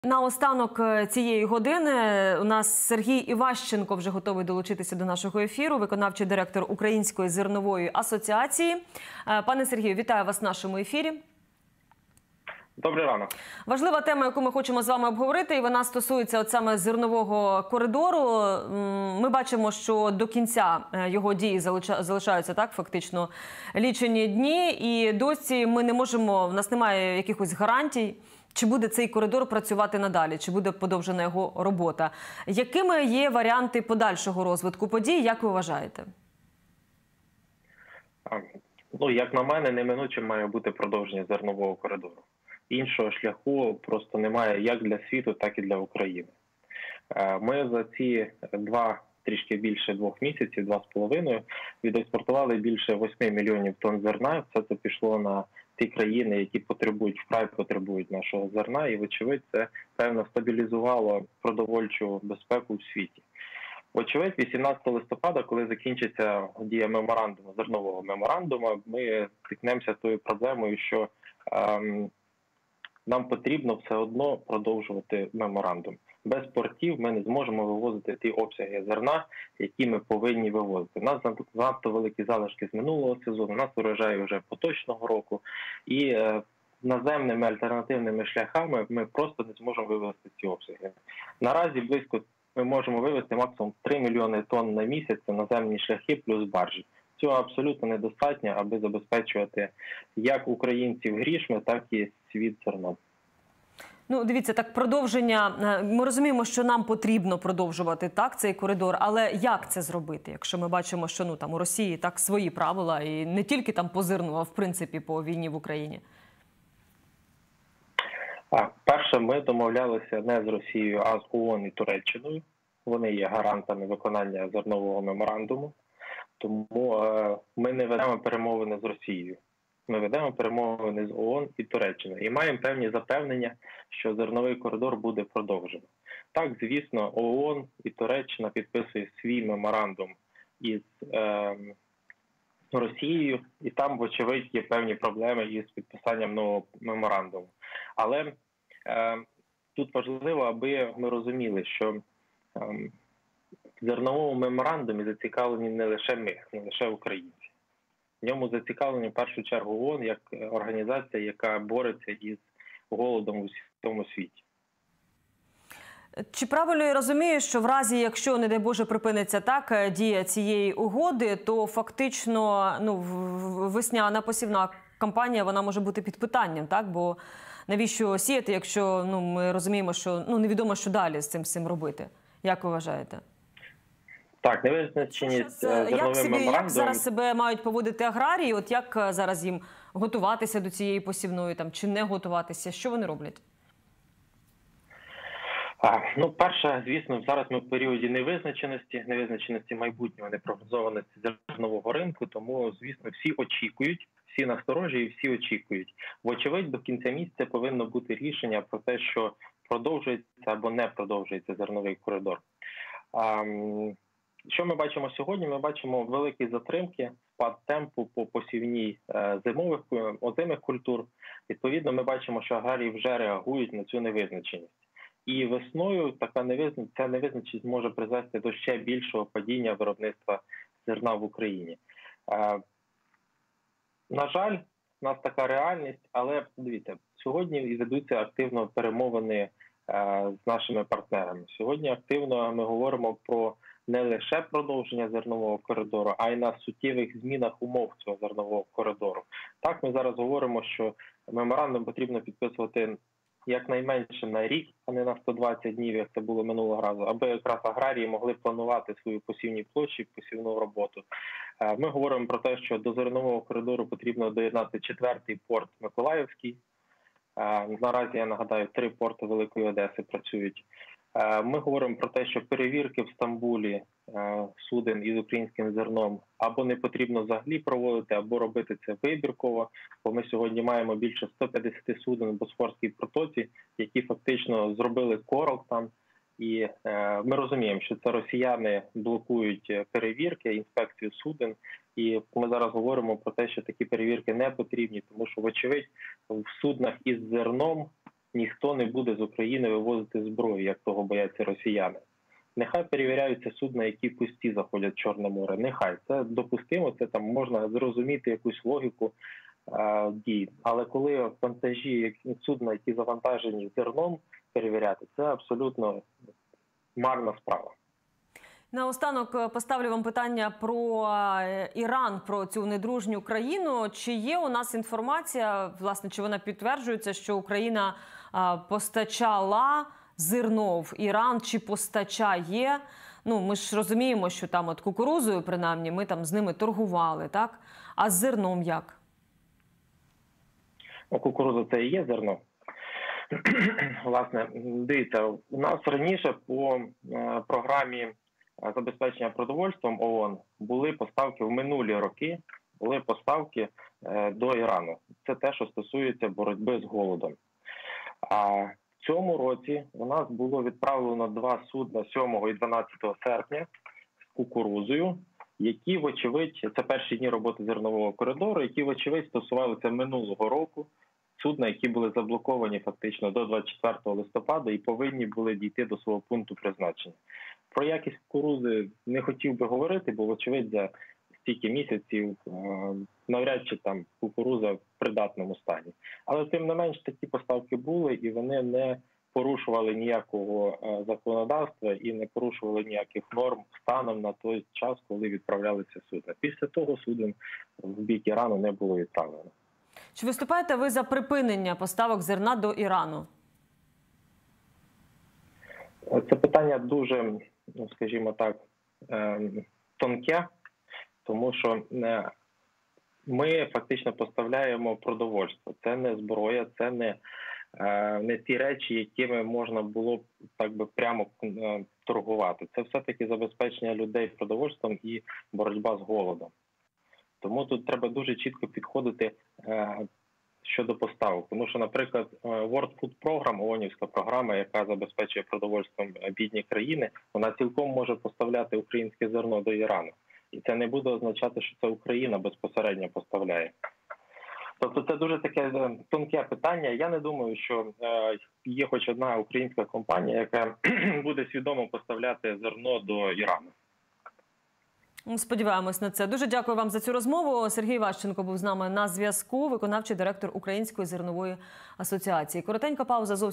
"Зернова угода" має бути продовжена, іншого шляху немає – Сергій Іващенко
Сергій Іващенко, виконавчий директор української зернової асоціації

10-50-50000 - будь-яка сума має значення.
Разом переможемо!
Слава Україні!